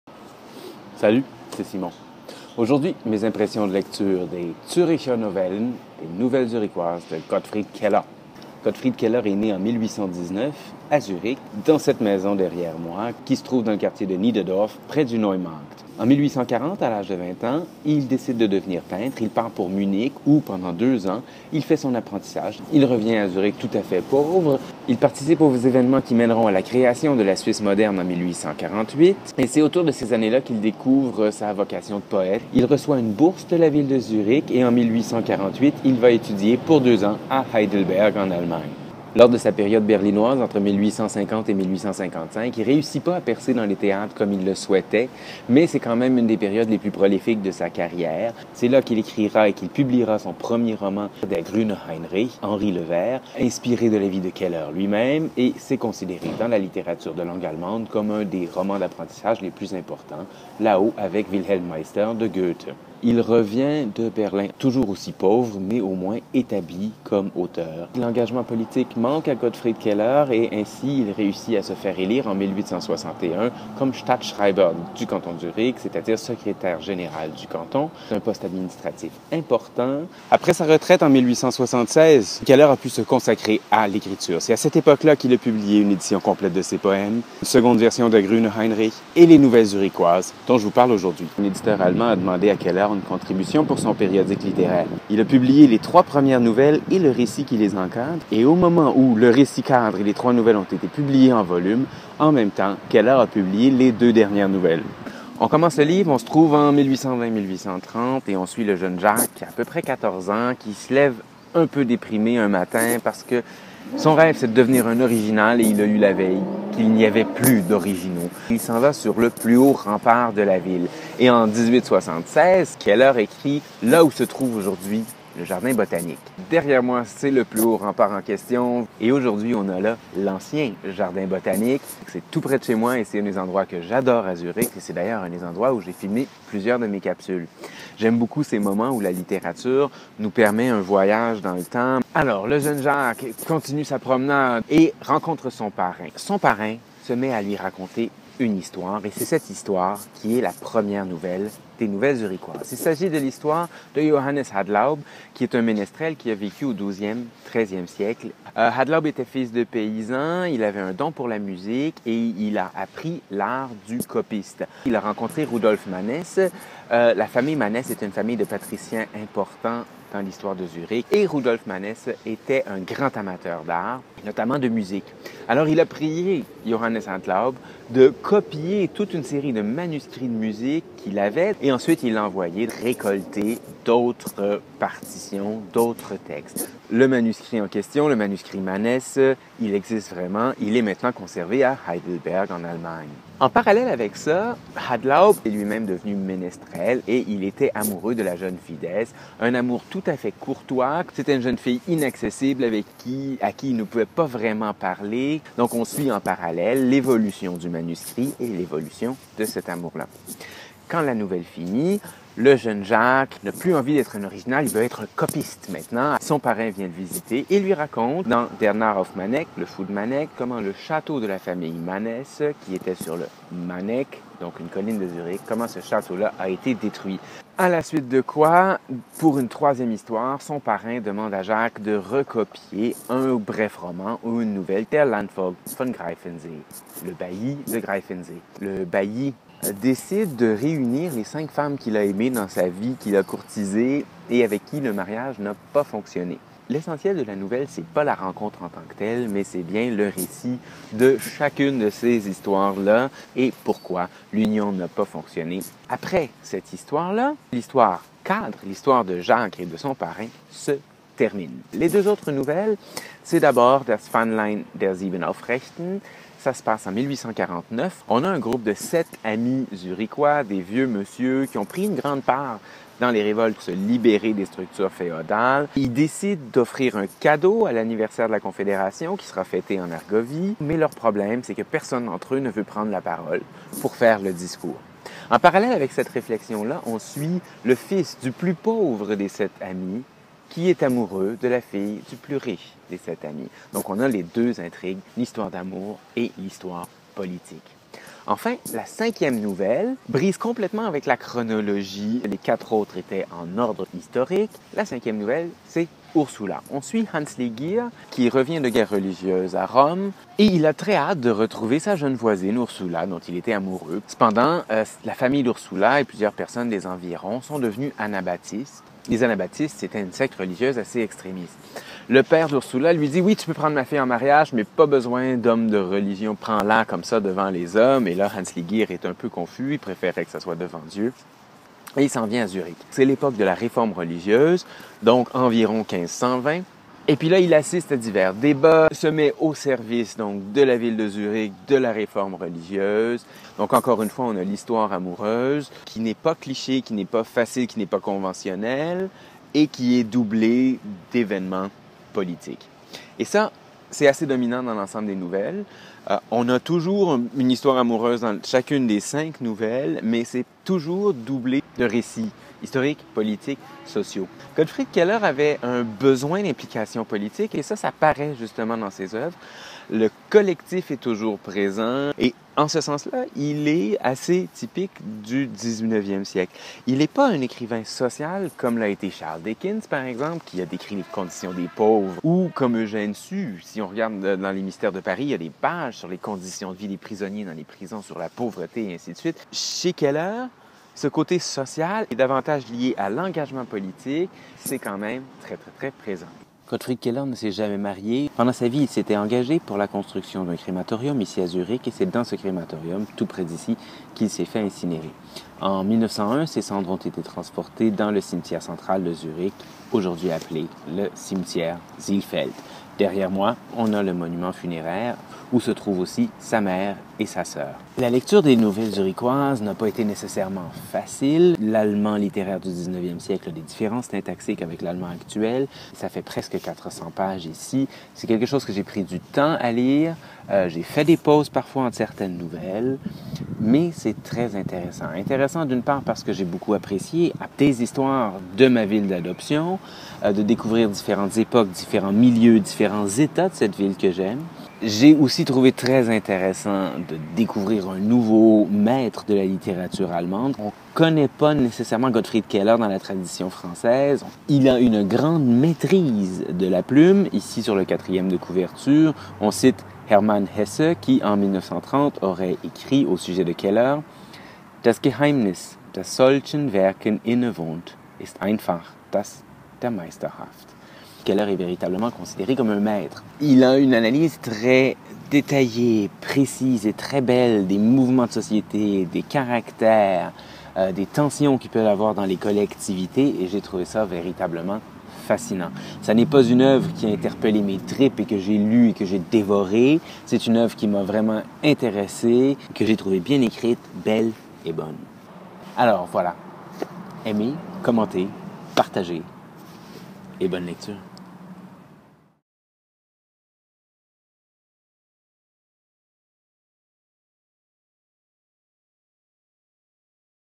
[28] Zurich au fil des siècles - Keller - Nouvelles zurichoises
Impressions de lecture de:
Keller, Gottfried. Züricher Novellen. Berlin, Insel Verlag, 1971 [1876].

Tourné à Zurich.
- Naissance de Keller: Rindsmarkt (Niederdorf);
- Monument à Keller: Enge;
- Fortifications:Schantzengraben, près du Alte Botanischer Garten;
- Fin: Cimetière Sihlfeld.